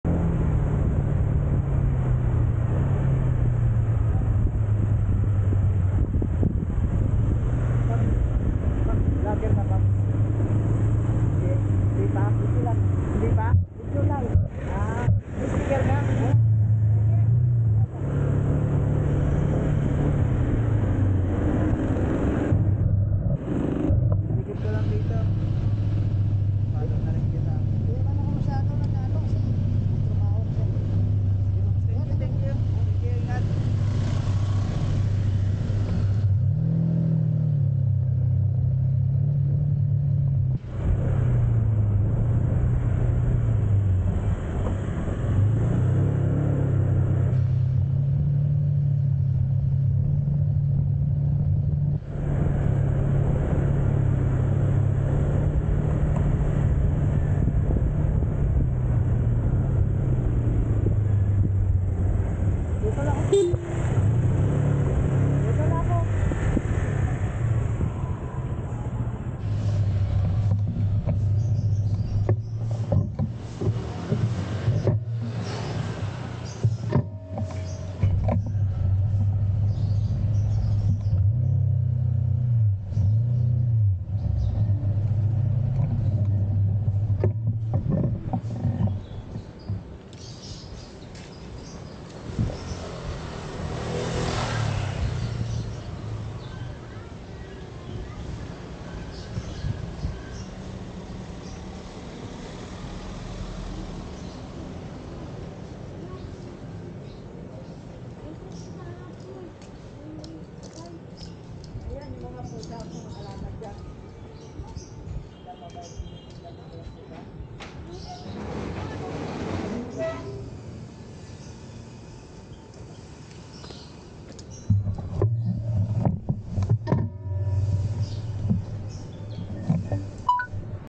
KALA KO ANO NA HINGI LANG PALA STICKER SI PAPS
habol Ng habol Si paps Kala ko ano na hinginlang Pala sticker